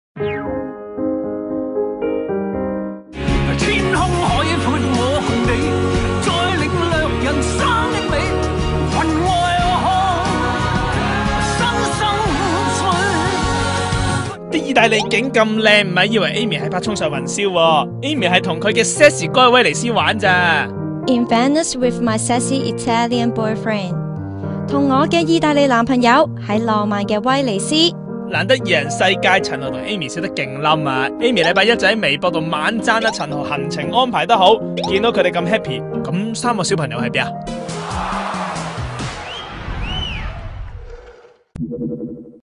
撇甩仔女 陳豪茵媺浪漫遊水都 - AC
同老公陳豪「三年抱三」嘅陳茵媺(Aimee)，4月嗰陣幫老公追到囡囡Camilla，不過Aimee就冇話過會封肚，隨時有機會四連中。

雖然要喺屋企湊小朋友，不過忙極都要唞唞氣，昨日Aimee就喺微博貼咗張同老公仔陳豪，喺「水都」威尼斯嘅相，仲好鬼馬咁話：「In Venice with my sexy Italian boyfriend...，同我的「意大利」男朋友在浪漫的威尼斯. 」